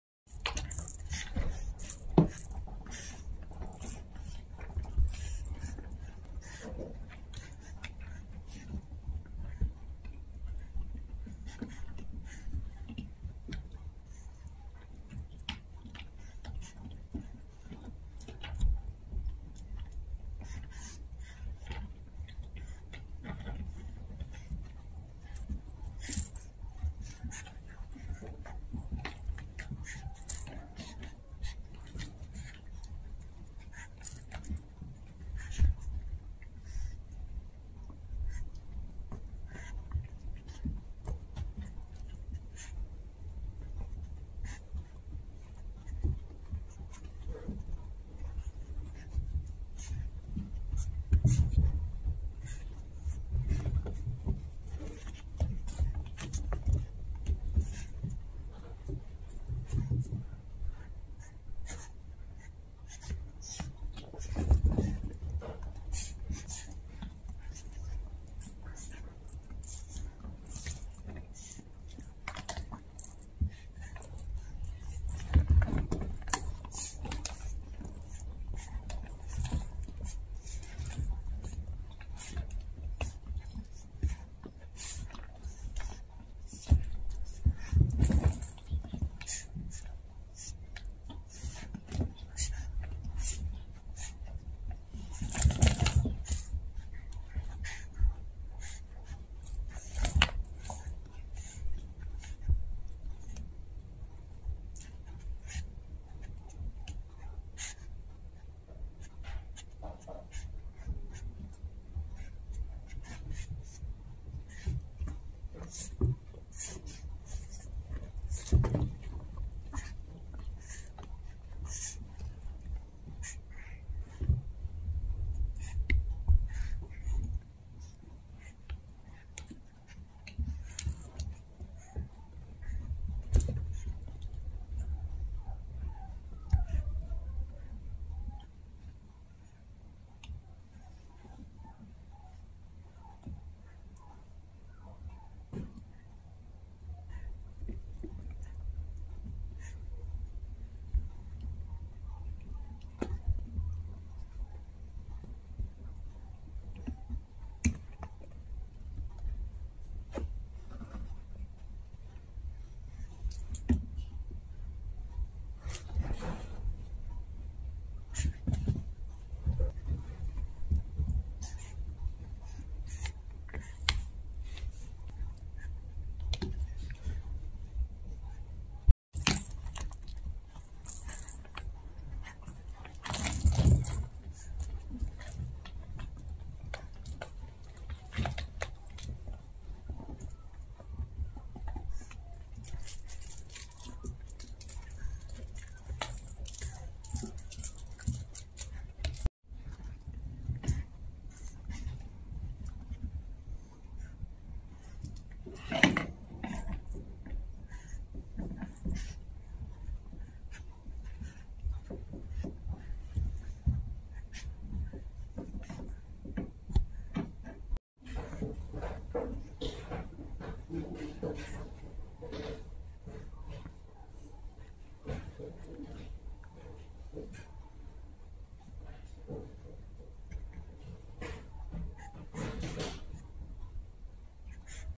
Chihuahua Teen Siblings' Playtime Extravaganza!
Join the fun as these lively Chihuahua teen siblings engage in a playful adventure, filled with boundless energy and sibling camaraderie. They are a blue sable Chihuahua male and a lilac tan Chihuahua female. Their antics are sure to bring a smile to your face. Don't miss this heartwarming display of puppy love and joy!